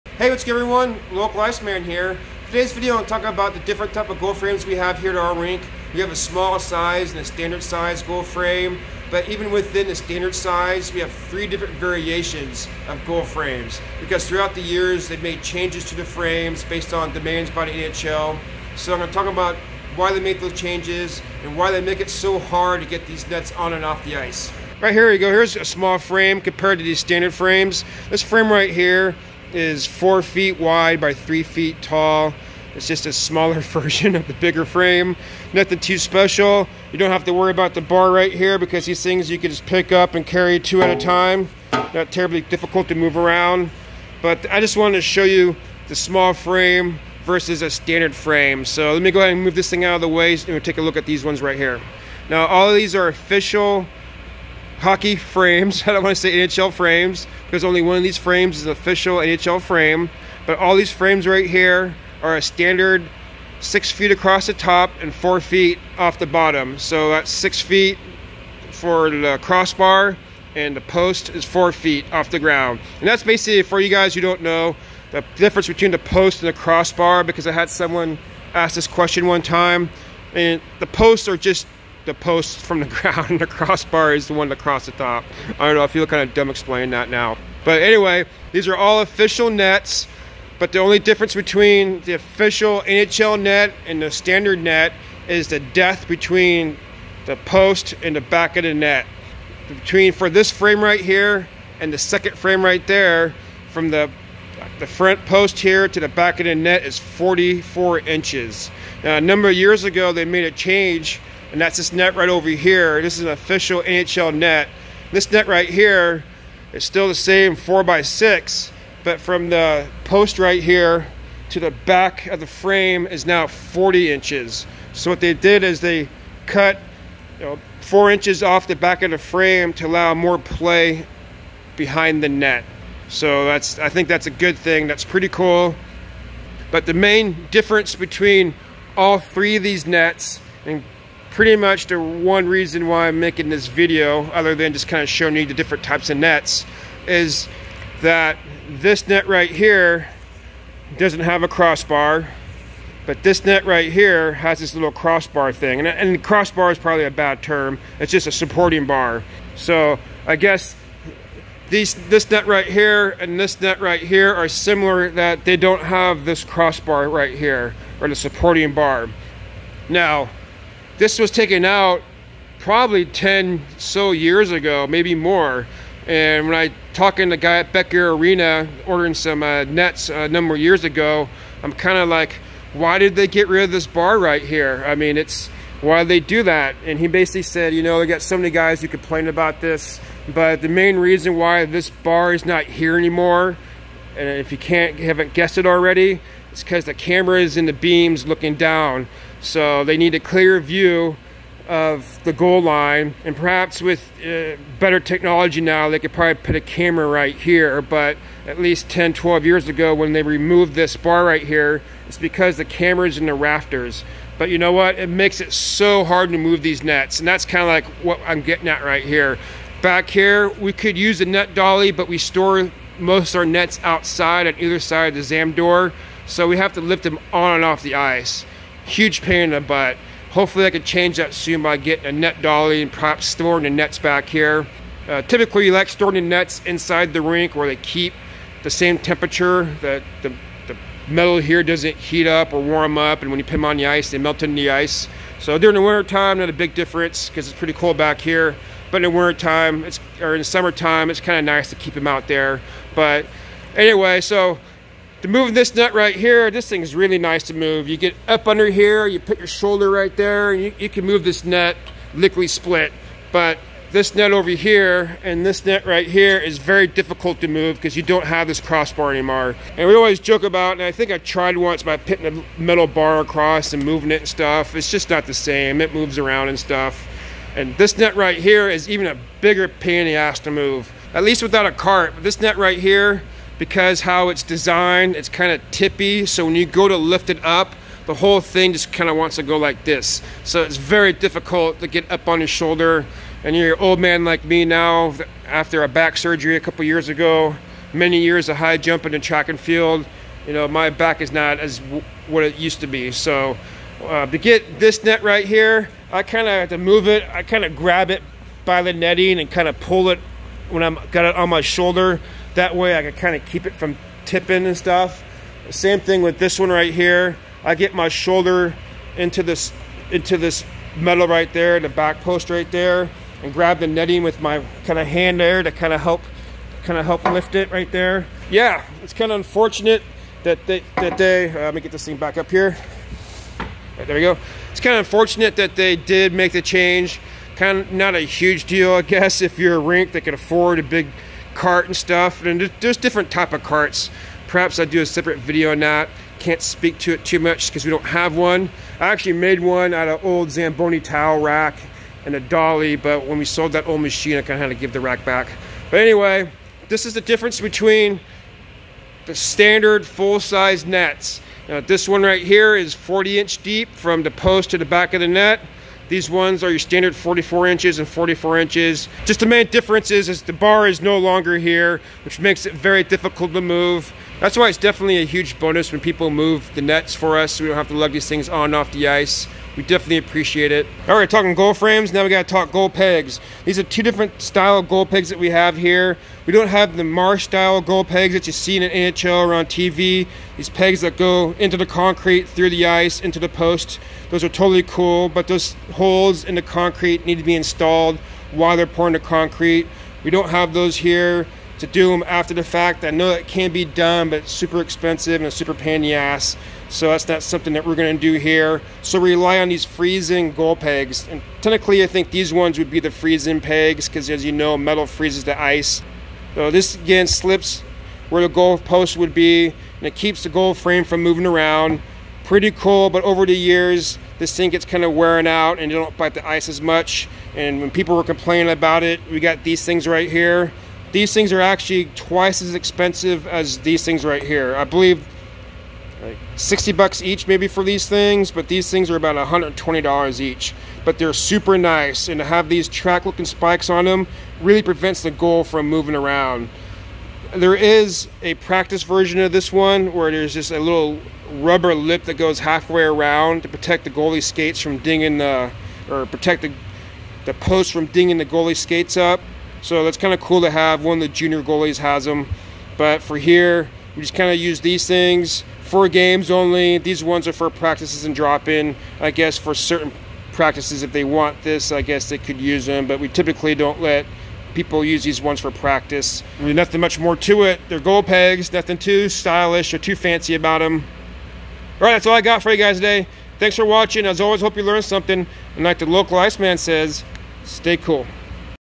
Hockey Goal Frames
Whats good everyone for this episode I do a video about the goal frames that we have at our rink. There are a few other styles of frames but I will focus on the ones that we have. I will do another video about netting and the pads at some point.

* I measured 17.5 and one would think it would have been 4, the same as the bottom. But the back top bar is only 3/4 now not 1 1/4 inch on the older nets. Perhaps that is the difference.

Questions and comments are much appreciated.

Your Local Ice Man